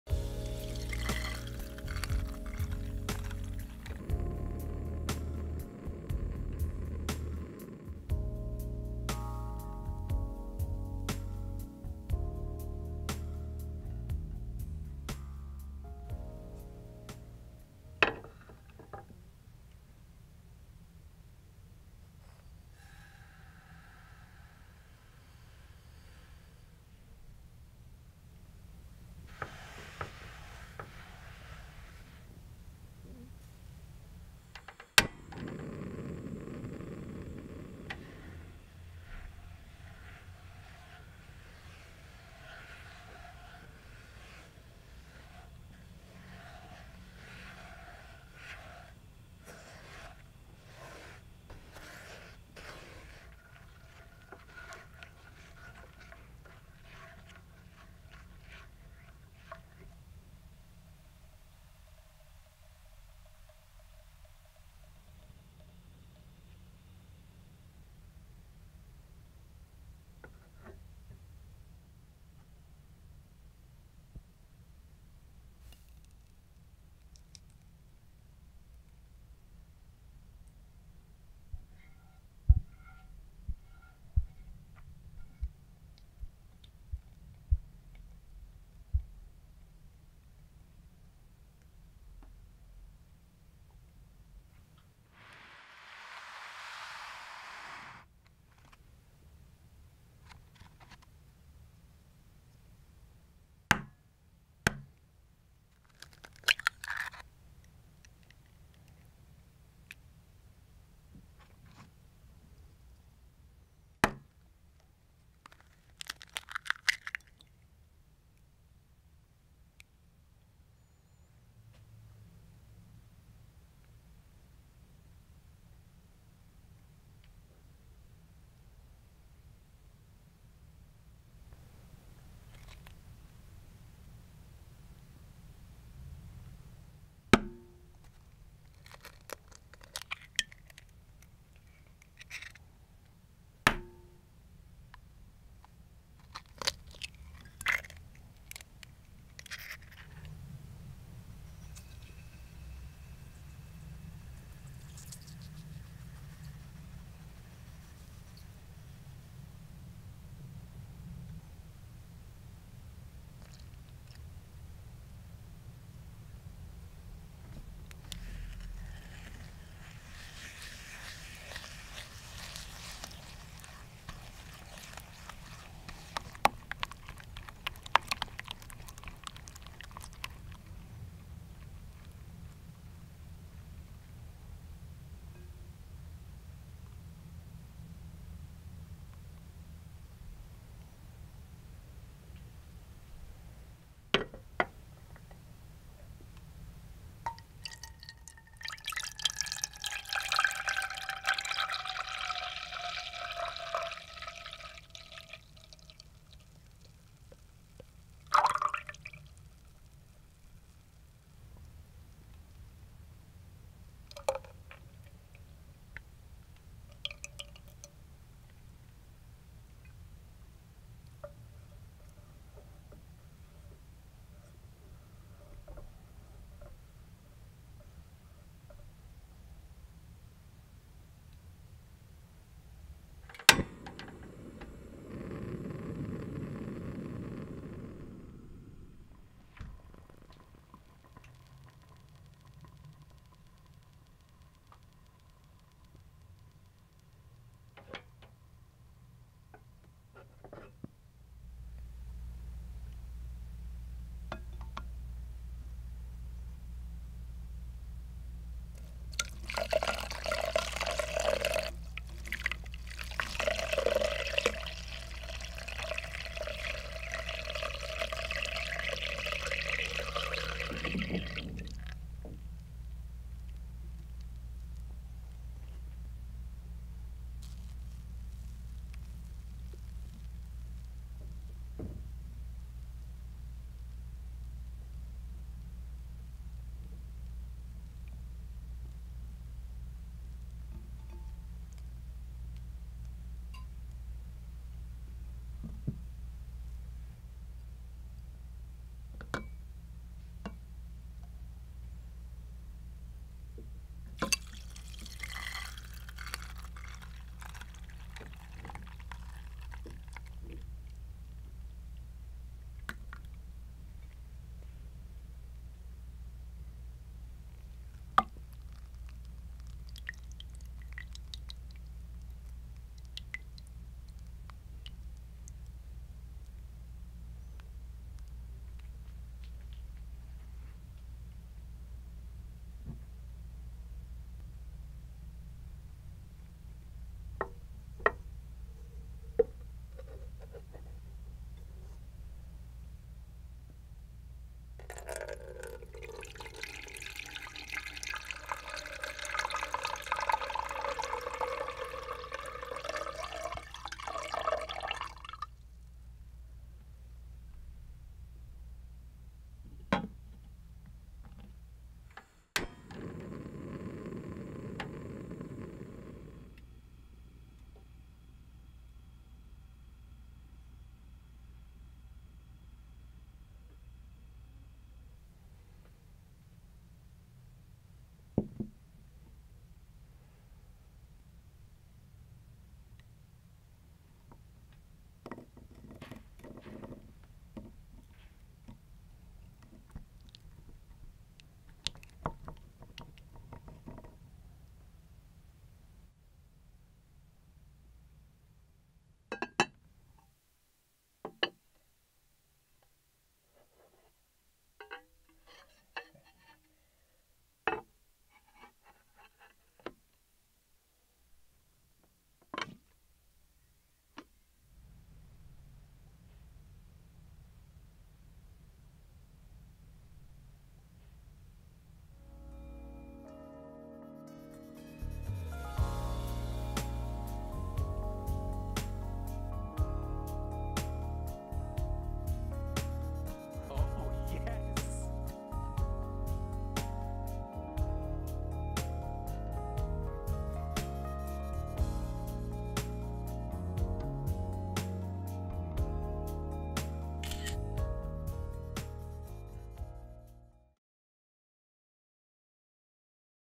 ASMR Baking: Rich & Creamy Flan • Tasty
For the most optimal experience, use headphones. Sit back, relax, and enjoy this rich and creamy flan.

MUSIC